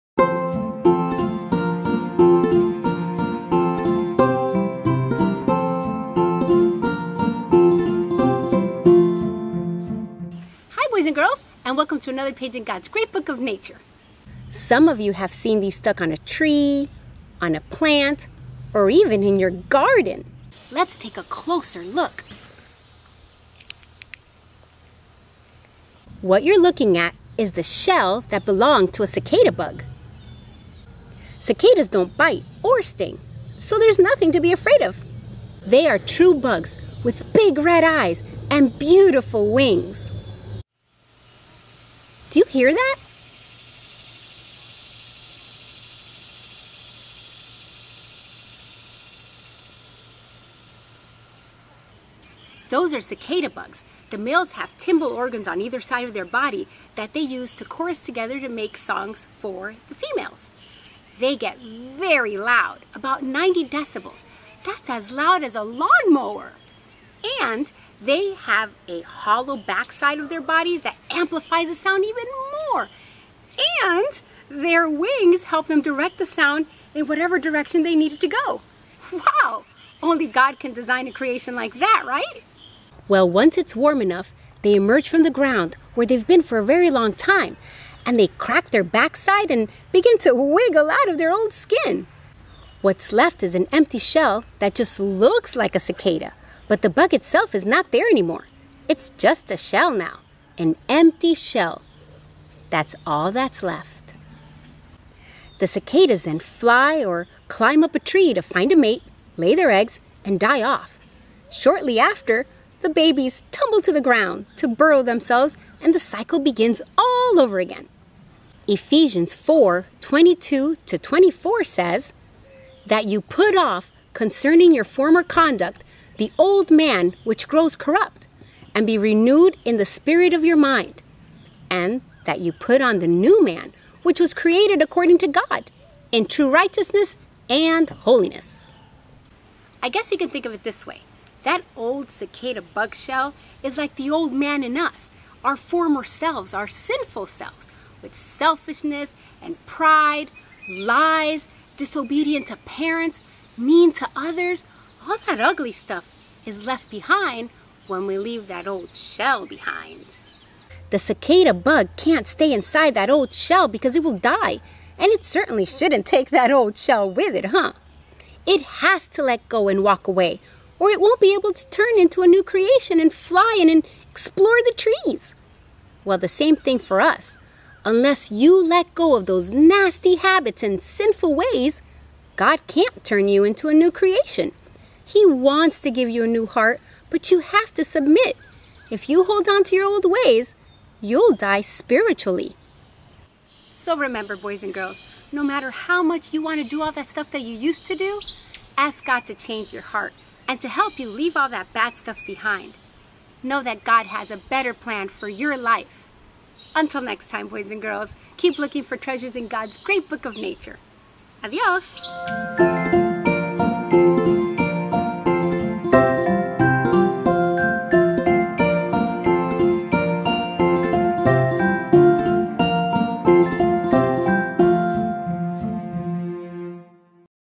Sharing Time: Cicadas Leave Old Self Behind
Look for our WEEKLY sharing time episode uploads on Wednesday and Friday at 9am EST!!!

This sharing time episode is a segment from our weekly Sabbath School program.